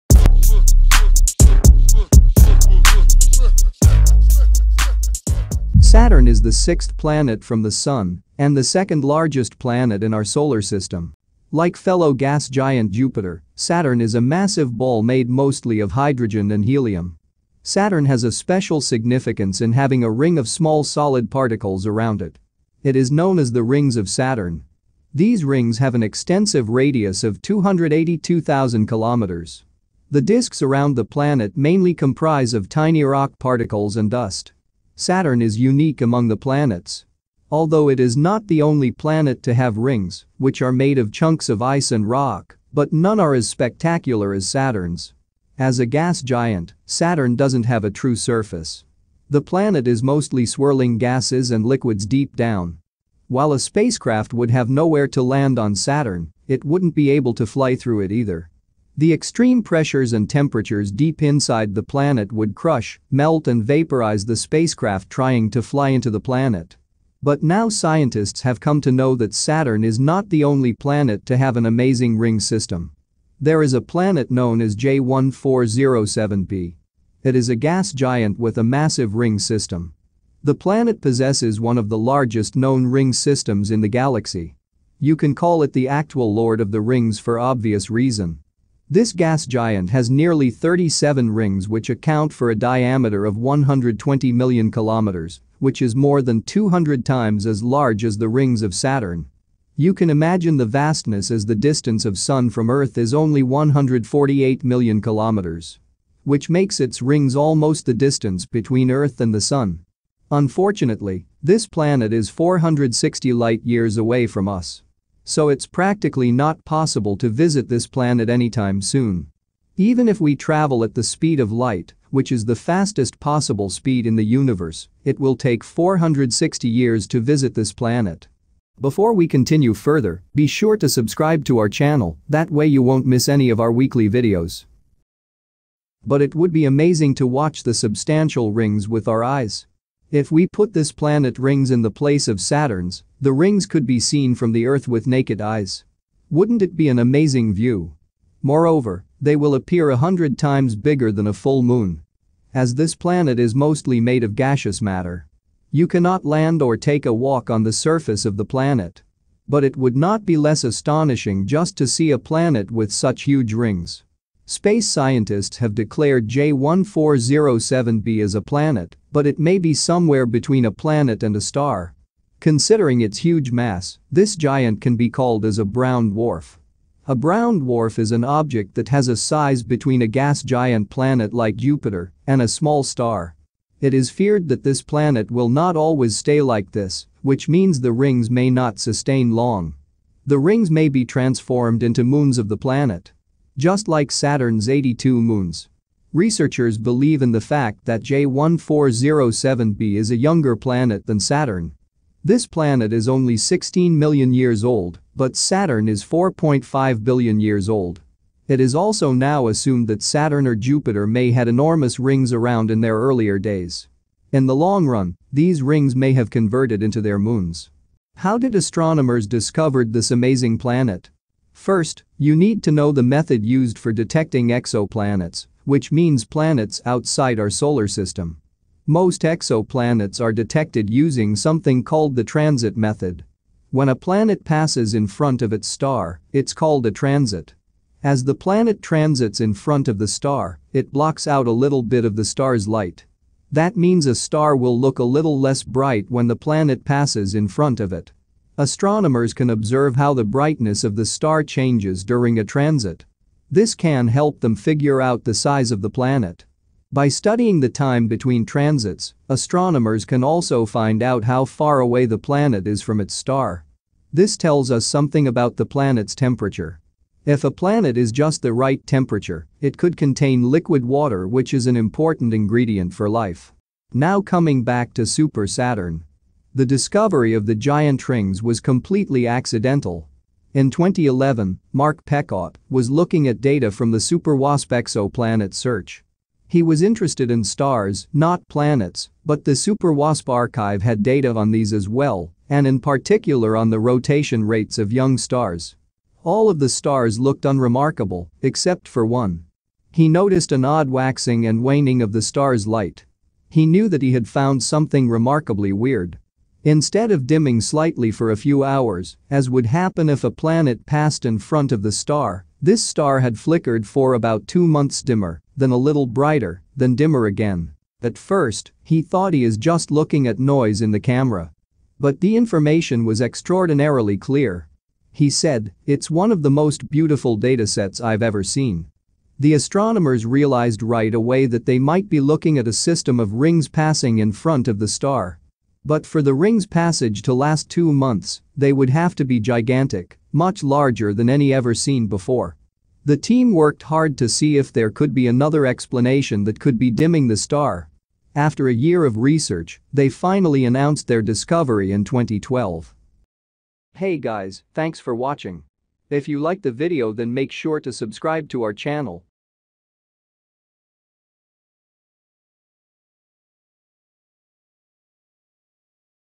Visiting Super Saturn 'J1407b' Planet
Astronomers have discovered an exoplanet ring system that puts Saturn's rings to shame.

There is a planet known as J1407b. It is a gas giant with a massive ring system. The planet possesses one of the largest known ring systems in our galaxy.

You can call it the actual ‘Lord Of The Rings’ or 'Super Saturn'. This gas giant has nearly 37 rings which account for a diameter of 120 million kilometers, which is more than two hundred times as large as the rings of Saturn.

You can imagine the vastness as the distance of Sun from Earth is only 148 million kilometers.

Unfortunately, J1407b exoplanet is 460 light-years away from us. So it's practically not possible to visit this planet anytime soon.

Space scientists have declared J1407b as a planet, but it may be somewhere between a planet and a star. Considering its huge mass, this giant can be called as a brown dwarf. A Brown dwarf is an object that has a size between a gas giant planet like Jupiter, and a small star.

It is feared that the planet J1407b will not always stay like this, which means the rings may not sustain long. The rings may be transformed into moons of the planet. Just like Saturns 82 moons.

Researchers believe in the fact that J1407b is a younger planet than Saturn. It is also now assumed that Saturn or Jupiter may have enormous rings around in their earlier days. In the long run, these rings may have converted into their moons.